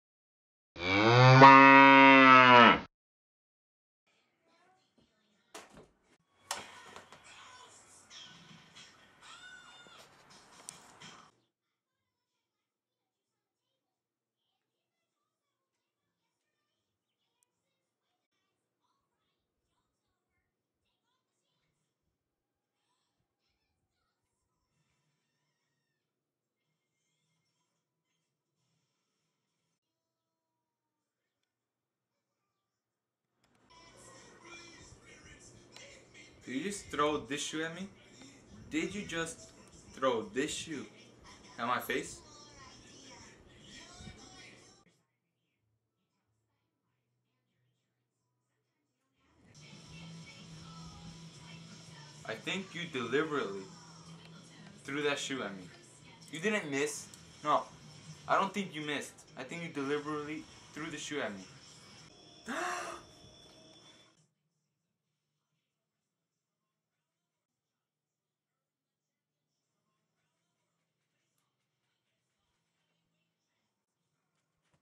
Shoeface
Samuel takes off his shoe but "accidentally" hits Roygin.